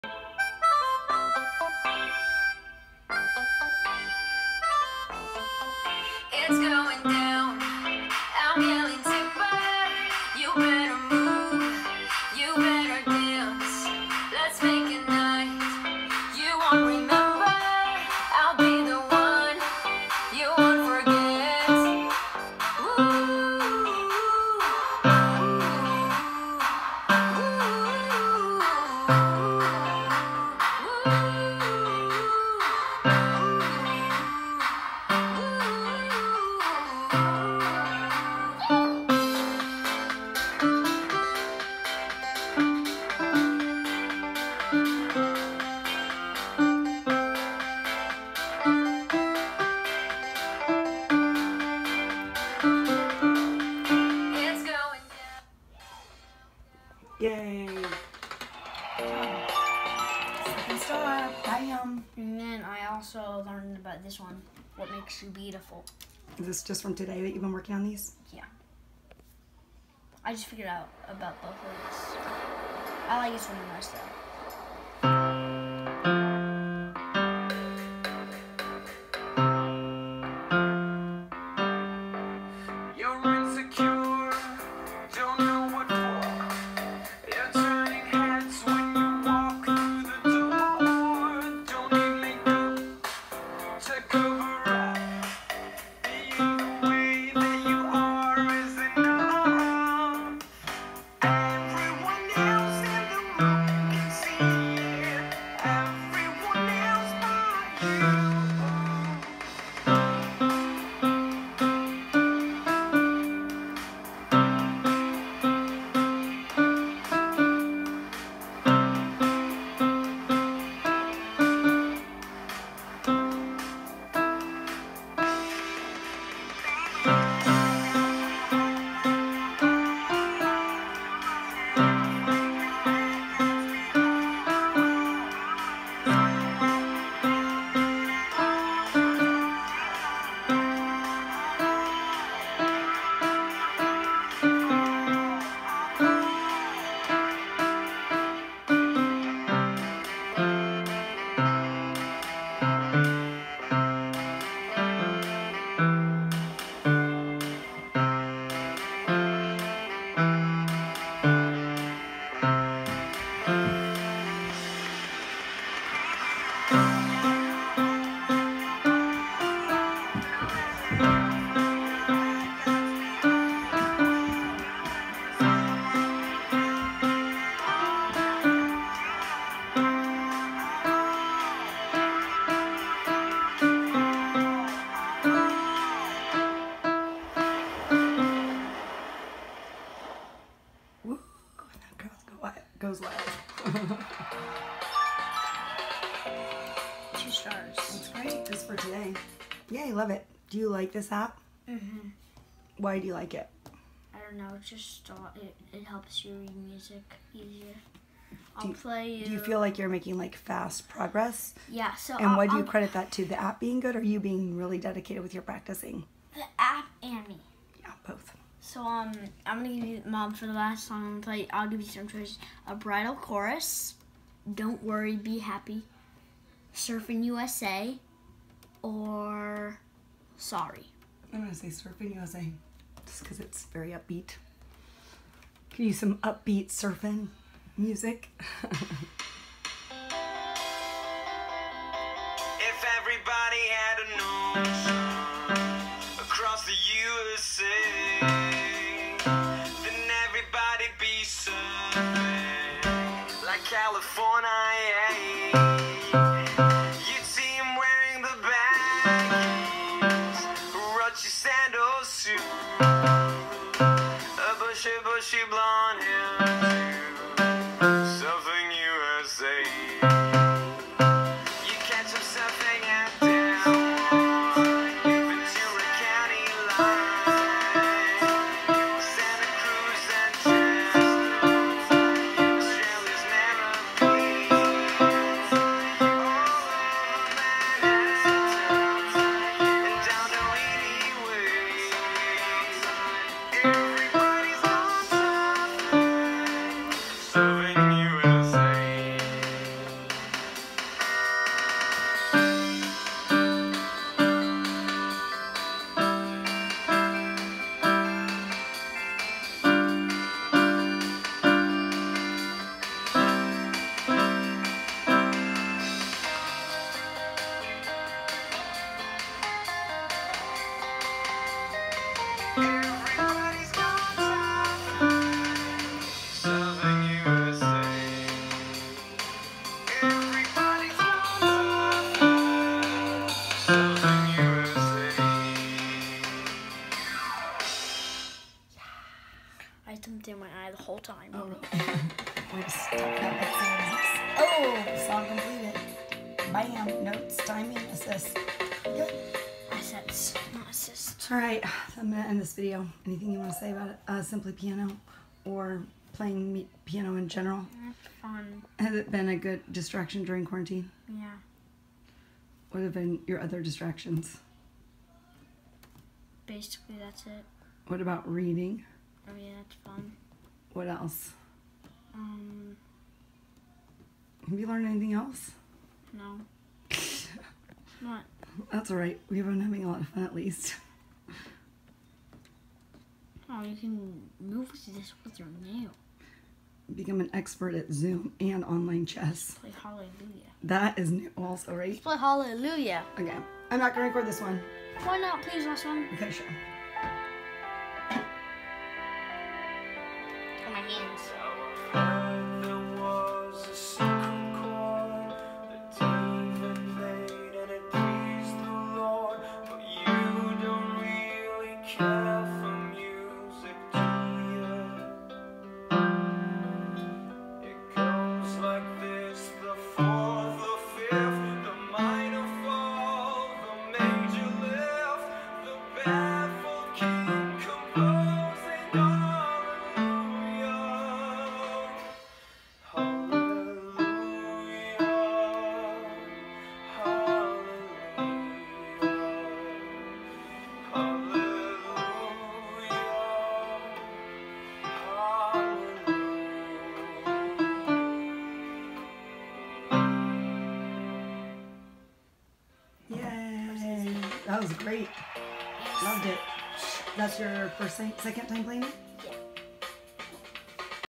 Quarantine Blessings: Maddox learning to play piano using SimplyPiano App
Maddox loves this app! He spends about 15-30 min a day practicing without complaining. Most often he motivates to practice himself but if he is ever wandering around looking for something to do, I ask him if he’s practices today and he smiles and says “oh great idea” and rushes off to practice. The app is so user friendly, I literally just installed it and gave him the iPad and he took it from there!! So proud of his progress and so impressed with him using both hands and just his overall growth in skills and devotion to learning. ❤️